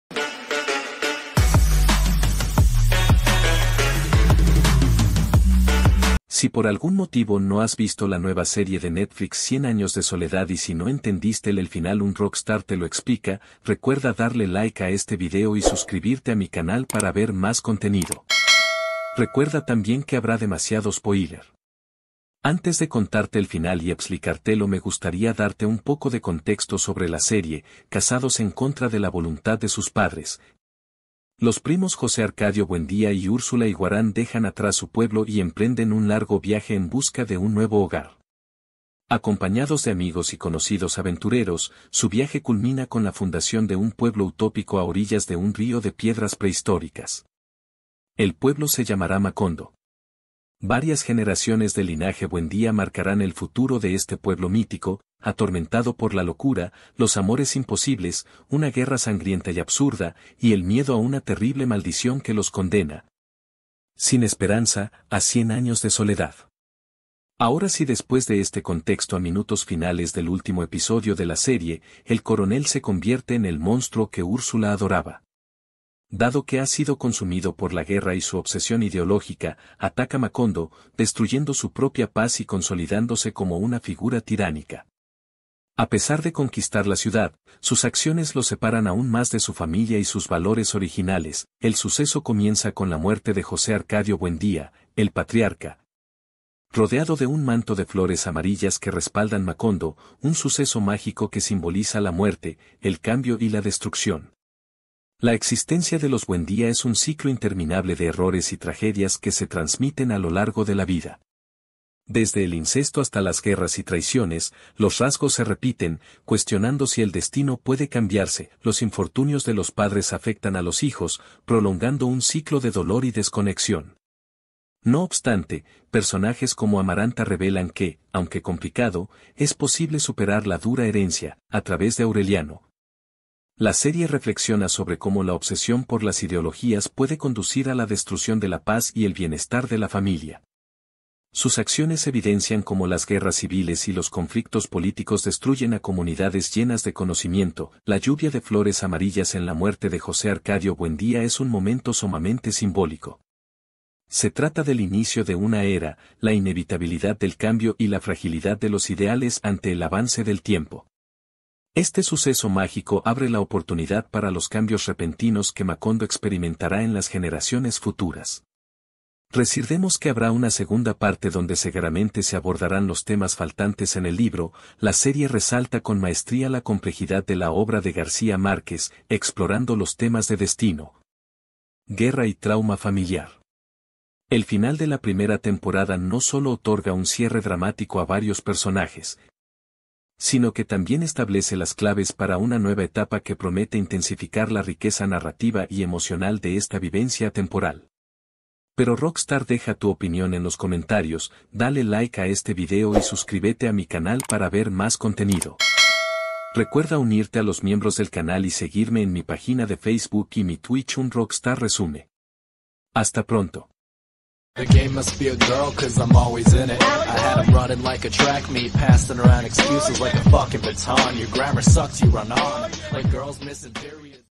CIEN AÑOS DE SOLEDAD - FINAL EXPLICADO (SERIE NETFLIX)
Analizamos el final de Cien Años De Soledad para traerte una explicación clara y concisa. Si te quedaste pensando en ‘¿qué acaba de pasar?’, este es el video para ti.